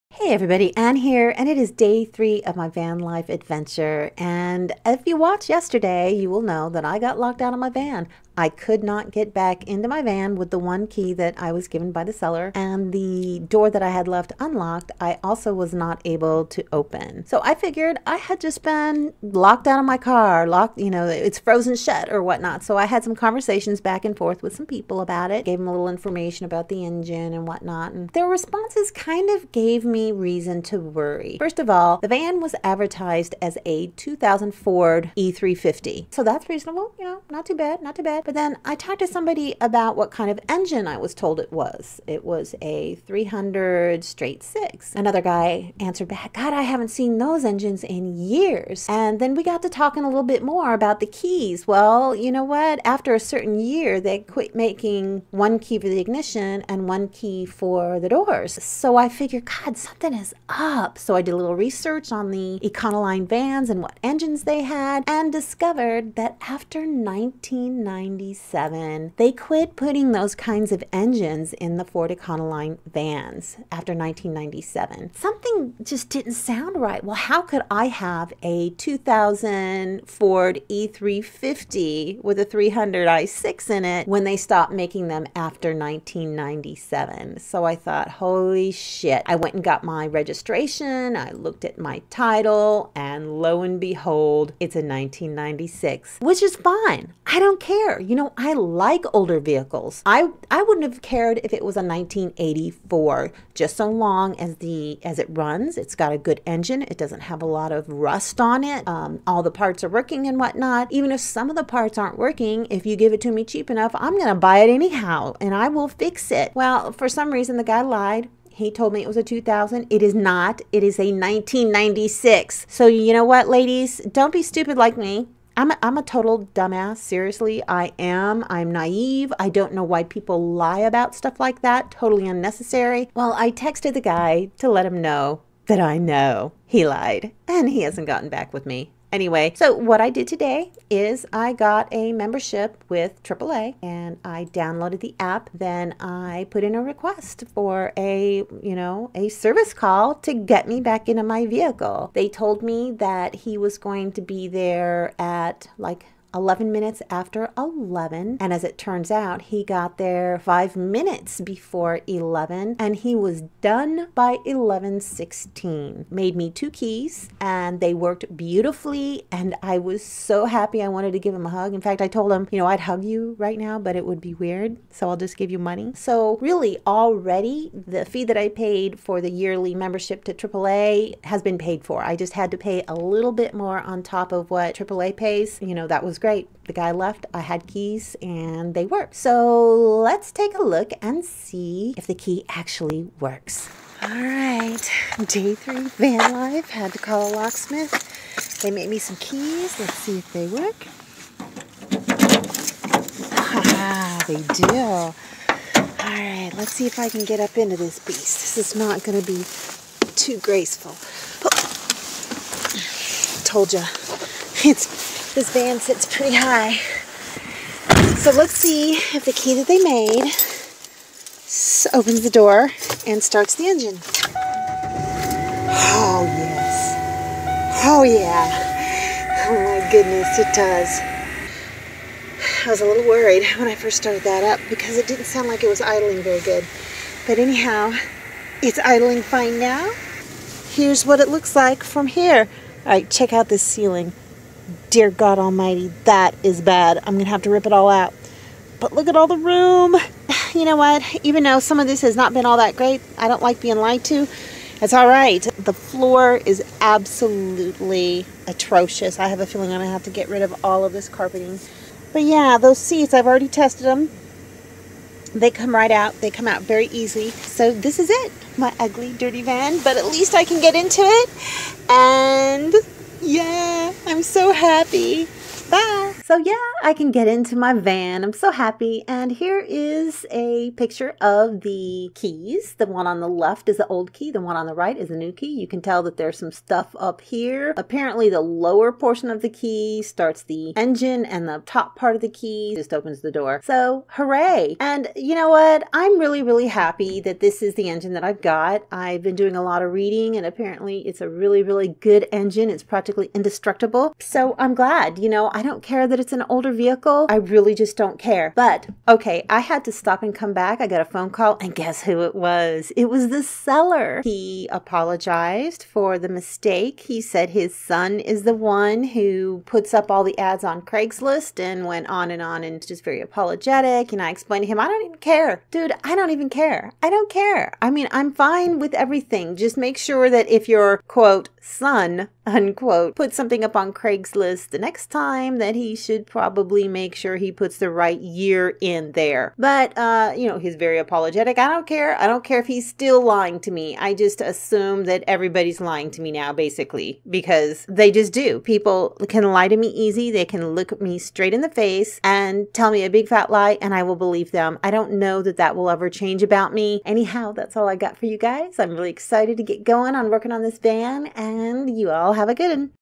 Locksmiths RULE! - Solo Female VANLIFE
Much thanks to Suburbia Pheonix who offered me many suggestions and support while I was figuring this all out.

The seller did end up calling me while I was editing the video so watch towards the end to see what he said.

Help me by clicking on the following link when making Amazon purchases. I'll get a small amount with each purchase you make!"